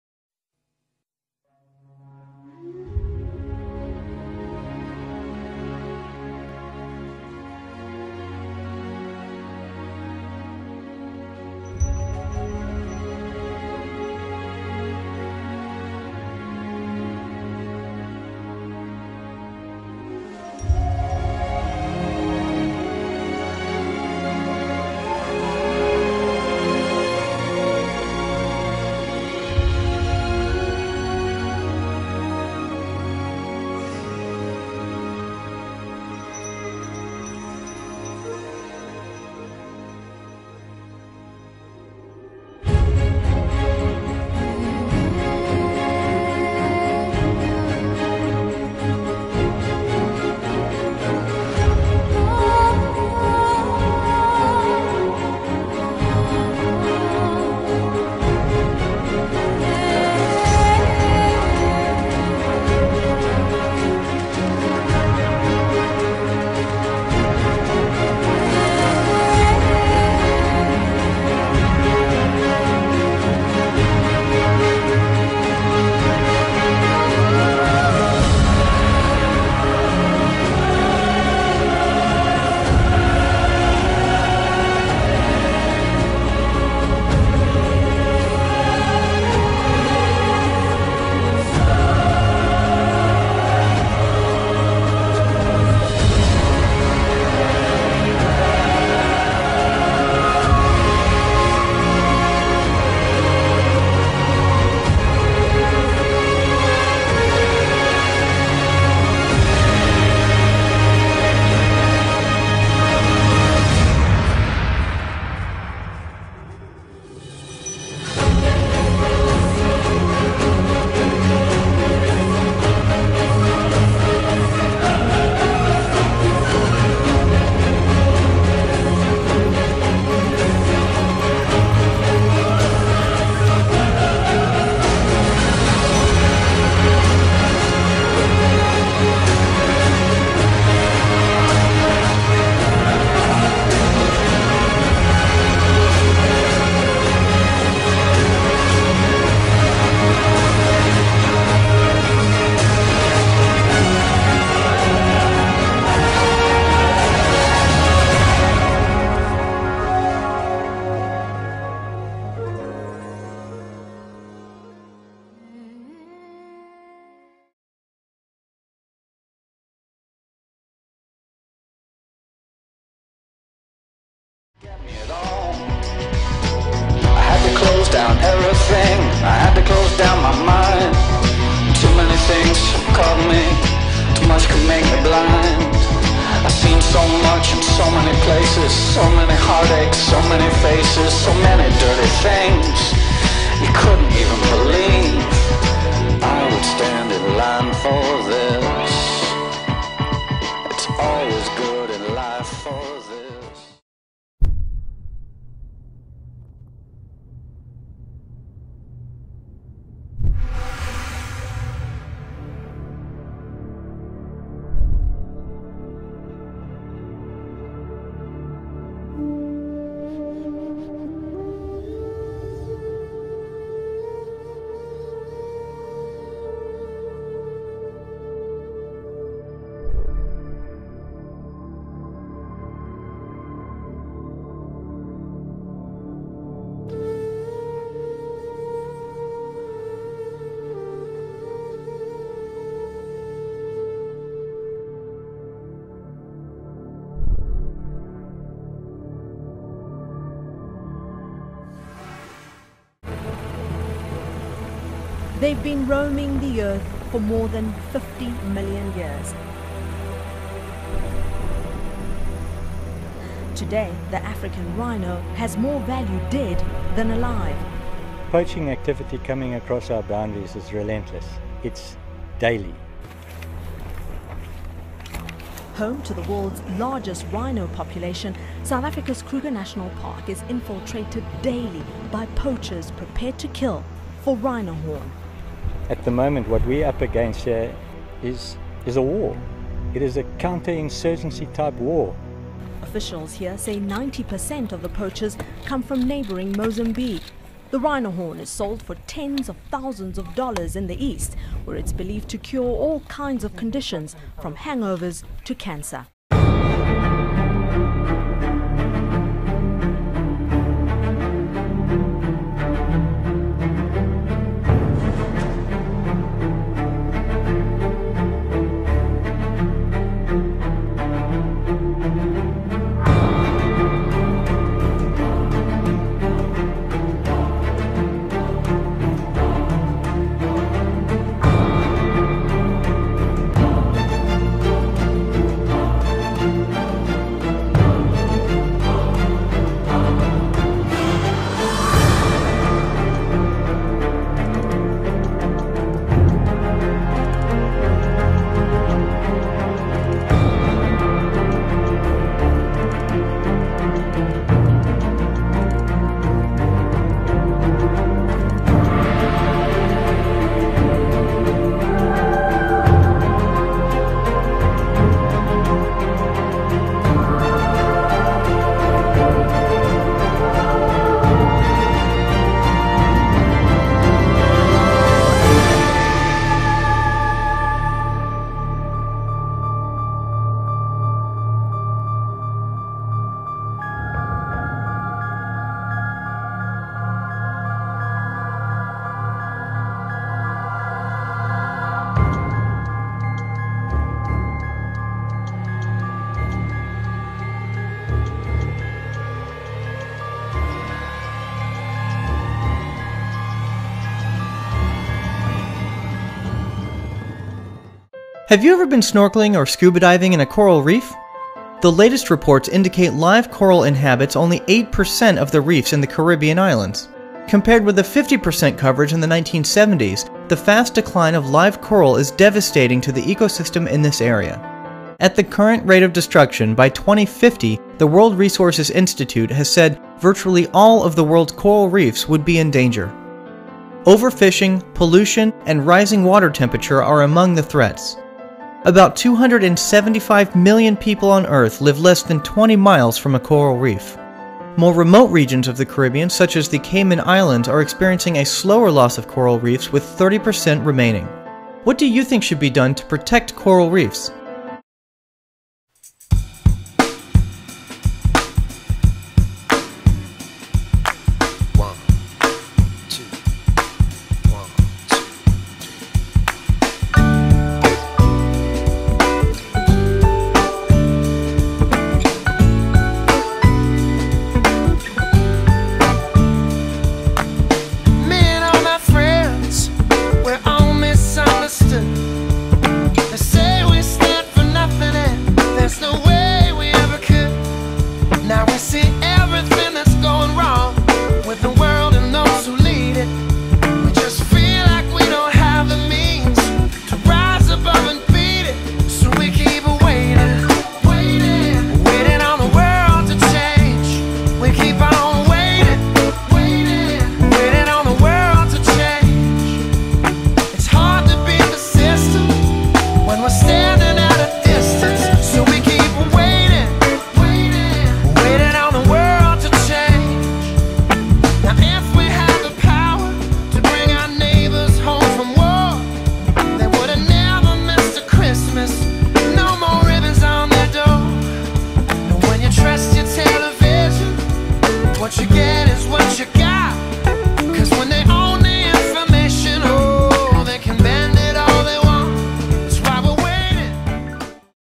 Ecosystems PBL Project #2
This video is a compilation of videos that provide our 6th Grade PBL students with an "entry event," for our upcoming Ecosystems Project. Driving Question: What Impacts Our World?